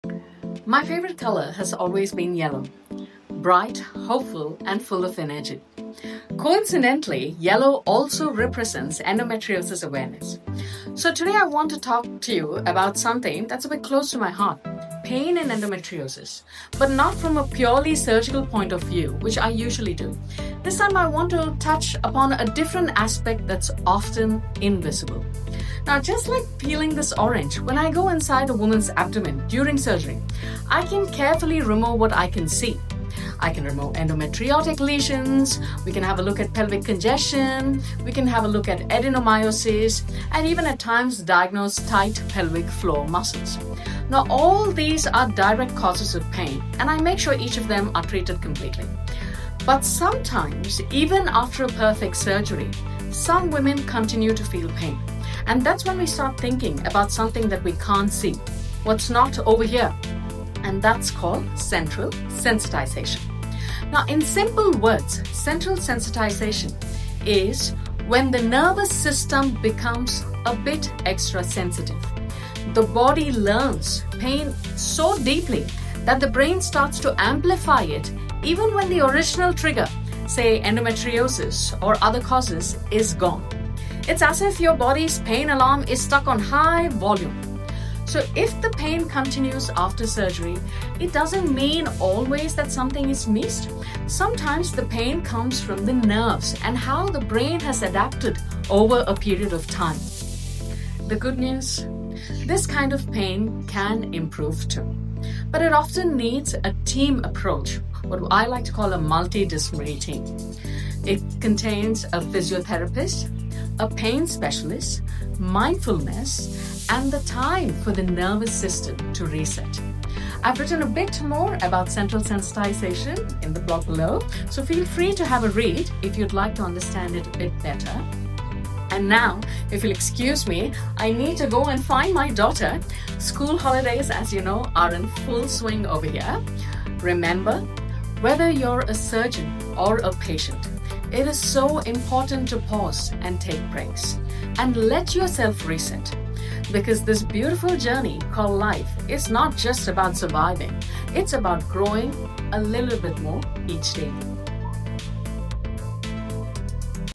Central sensitization in endometriosis
🌼 Pain, Endometriosis & The Hidden Layer We Don’t See 🌼

Yellow has always been my favourite colour — it’s bright, warm, and full of hope 💛
It also happens to represent Endometriosis Awareness, which feels fitting today, because I want to talk about a side of endometriosis that often remains invisible — Pain, and more specifically, why some pain continues even after surgery.

When I operate, I look for everything that can directly cause pain — endometriosis lesions, adenomyosis, pelvic congestion, tight pelvic floor muscles — and I make sure they’re all addressed. Most women feel better once these are treated.

But sometimes, pain persists even when everything looks perfect inside.
That’s when we start thinking about something called Central Sensitisation — a kind of neuropathic pain that surgery alone can’t fix.

🧠 What is Central Sensitisation?
Think of your nervous system like a sensitive alarm. Over time, chronic pain from endometriosis can “train” the brain to stay on high alert — even when the danger is gone.
The brain begins to amplify pain signals, so even normal sensations — like touch, bloating, or mild cramps — start to feel painful.
It’s not psychological. It’s physiological — your brain and spinal cord literally rewire themselves to become more sensitive to pain.

This is why some women continue to experience pelvic pain despite successful excision surgery. It doesn’t mean something was missed — it means the nervous system needs healing too.

Central sensitisation can be treated.
It just needs a different approach — one that combines physical, emotional, and neurological healing:
🌿 Pelvic physiotherapy
🧘🏽 Mindfulness and relaxation
💊 Medications that calm nerve pain
💬 Support from pain specialists and psychologists

Recovery from endometriosis isn’t always linear — and that’s okay.
Whether you’re a patient or a surgeon, it’s important to take breaks, reset, and remember that healing is as much about patience as it is about precision.